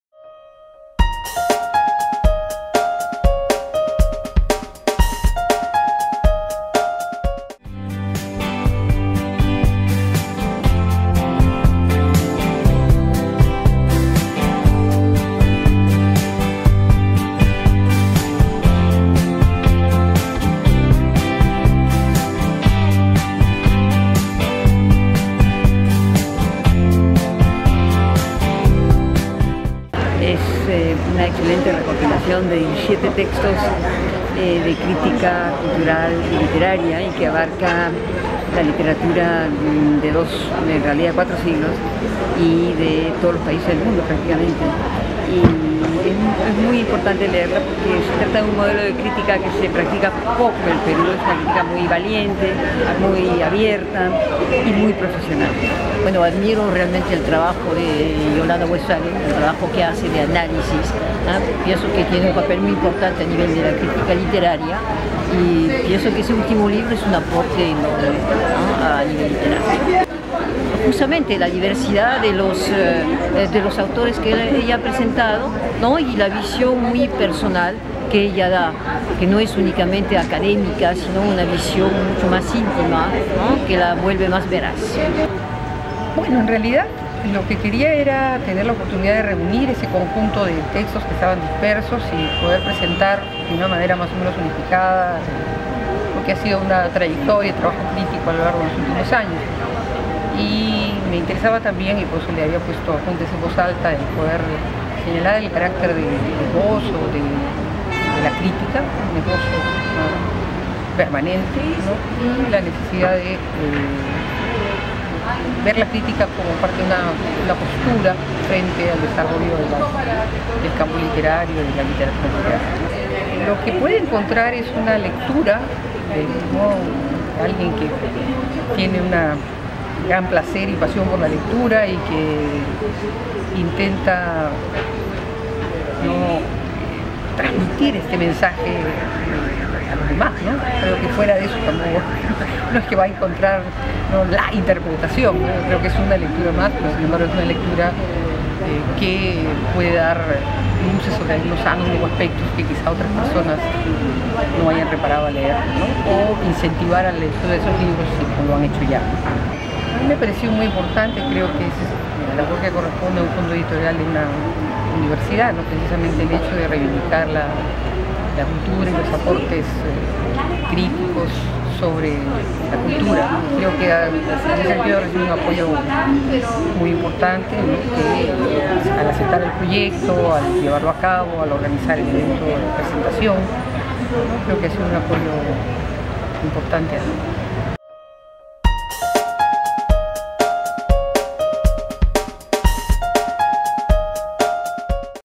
Presentación del libro "Apuntes en voz alta" de Yolanda Westphalen - 35º Feria Ricardo Palma
El pasado 27 de noviembre se presentó en la 35º Feria del Libro Ricardo Palma "Apuntes en voz alta" de Yolanda Westphalen, conjunto de ensayos sobre varias novelas de la literatura mundial, latinoamericana y peruana.

Se trata de una colección de 17 de textos que su autora logró reunir a lo largo de 15 años, donde se analizan reconocidas obras como el Ulises de James Joyce o Memorias de Adriano de Marguerite Yourcenar desde una nueva mirada crítica.